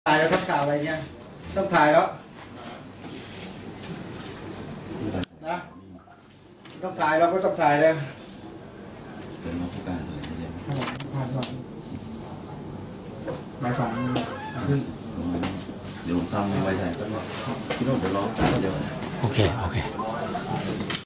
กองปราบฯ จับนายกฯ อบต.หนองกบ เรียกรับเงินฯ
13.30 น. 17 ก.พ. 58 ที่ กก.2 บก.ป. เจ้าหน้าที่ตำรวจ กก.2 บก.ป. ควบคุมตัวนาย.ประเสริฐ นุ่มรอด  นายก อบต.หนองกบ หนองแซง สระบุรี เรียกรับเงินเจ้าหน้าที่ อบต.ในการสอบเลื่อนวิทยฐานะ